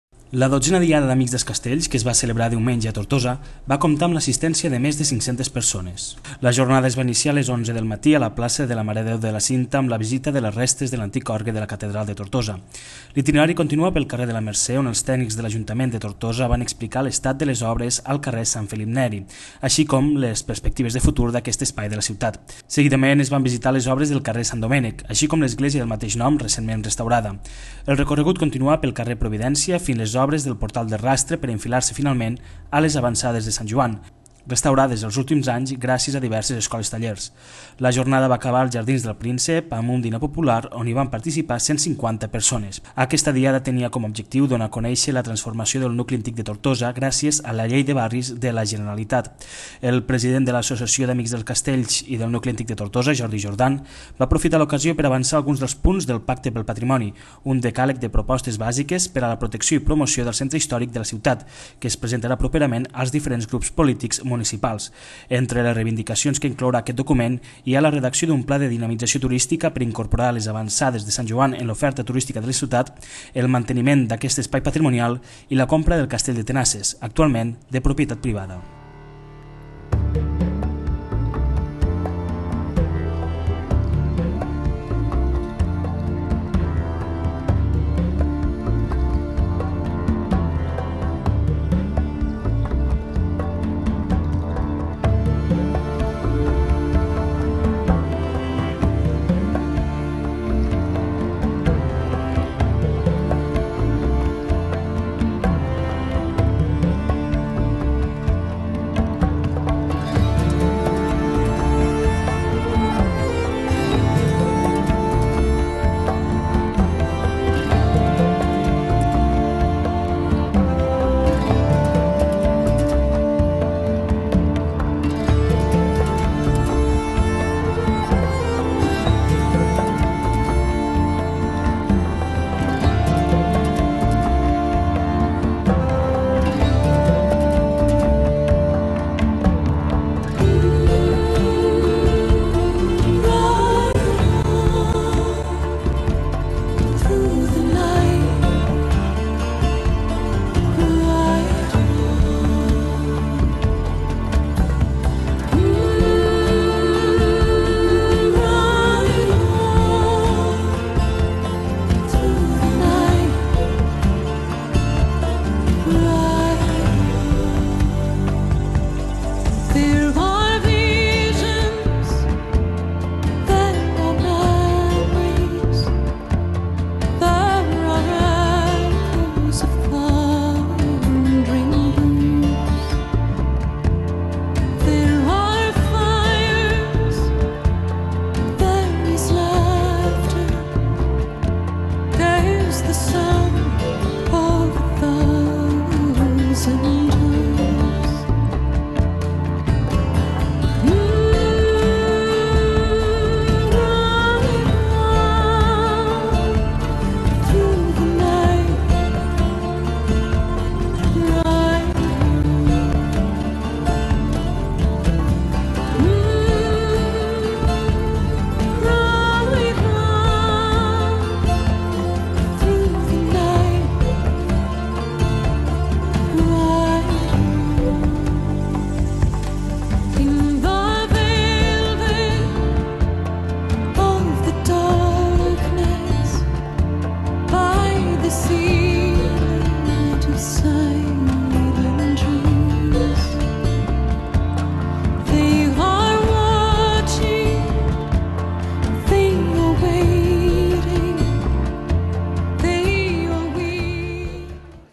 12a Diada d'Amics dels Castells
Més de 500 persones participen a la 12a Diada d'Amics dels Castells